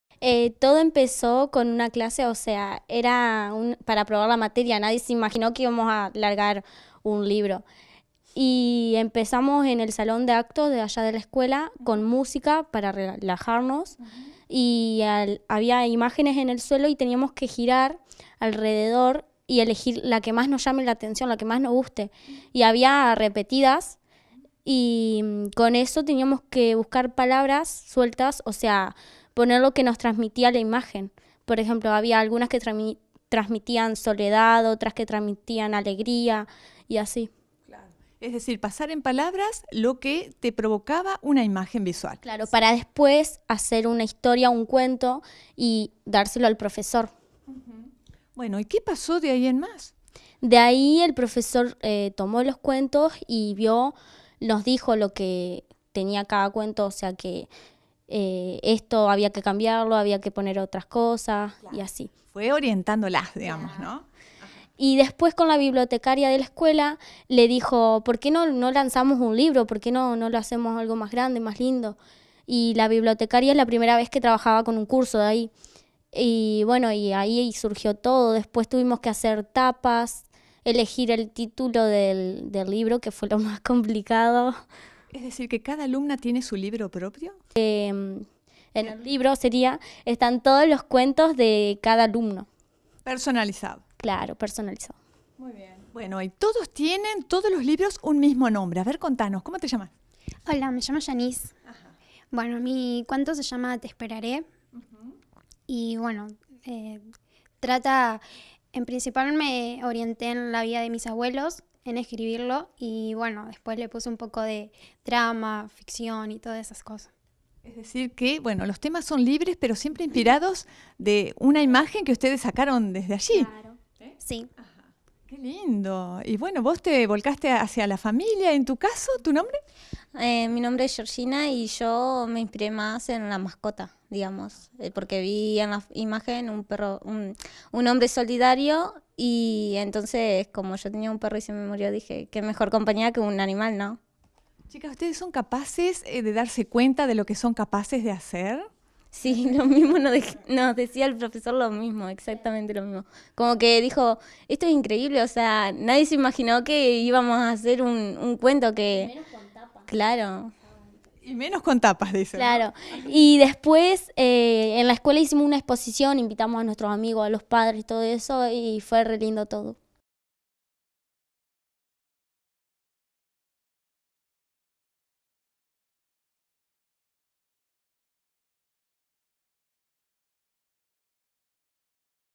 A partir de imágenes, adolescentes escribieron cuentos e hicieron libros
Alumnos de Quinto Año del Colegio Nacional Domingo Faustino Sarmiento de la capital entrerriana editaron sus propios libros con cuentos que surgieron a partir de imágenes. La actividad formó parte de la actividad desarrollada en la materia Literatura Latinoamericana.